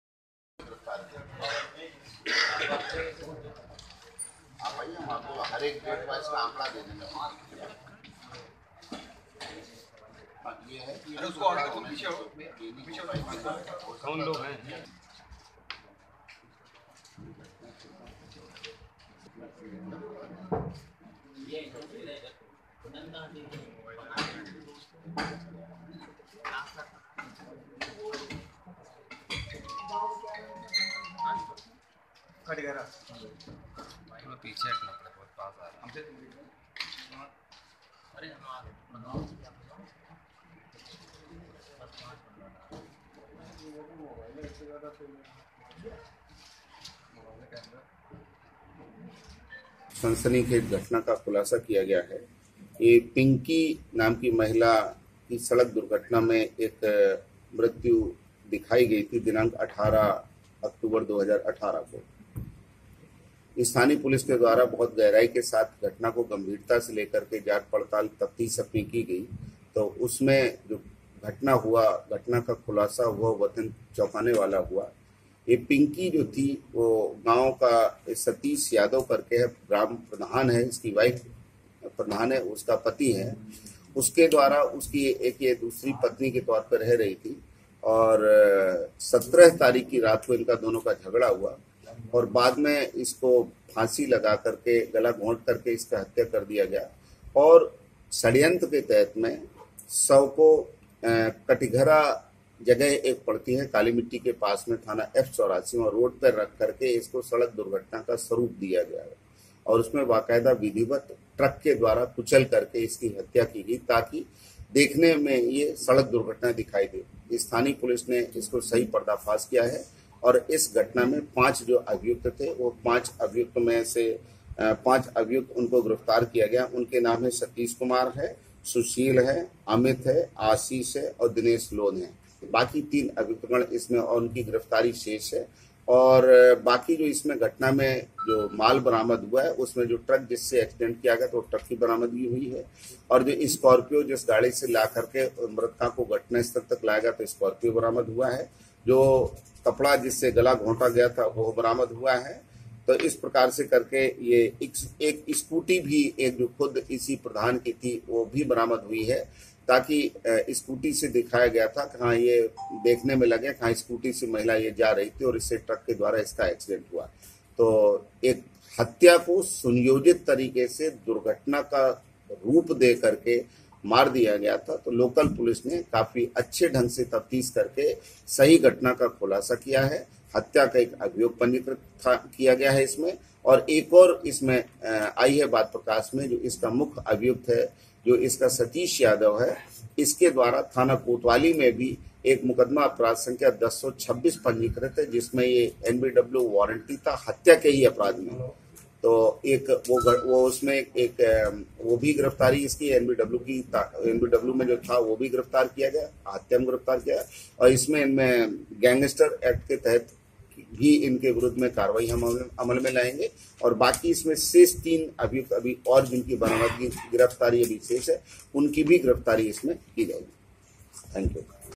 पति ने अपनी दूसरी पत्नी को उतारा मौत के घाट, पुलिस ने 5 लोगों को किया गिरफ्तार
उन्नाव: पति ने अपनी दूसरी पत्नी को मौत के घाट उतार कर सड़क दुर्घटना का रूप देकर पुलिस को चकमा देने का काम किया था. पुलिस ने प्रेस कांफ्रेंस कर मामले का खुलासा करते हुए पांच लोगों को गिरफ्तार कर जेल भेज दिया है.